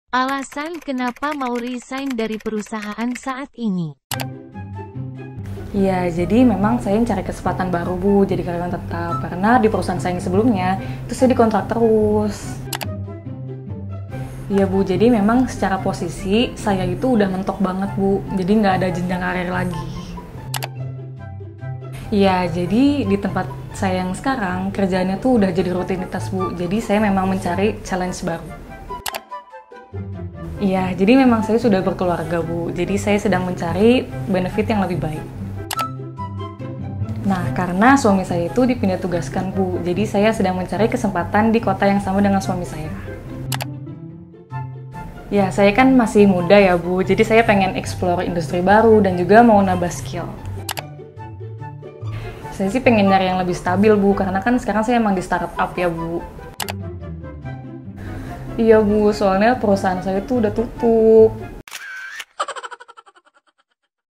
Alasan kenapa mau resign dari perusahaan saat ini
Jadi kemaren resign alesan lo kenapa nih? ada yang sama kayak yang disebut di video? 😄

Save dulu siapa tau nanti butuh ↗️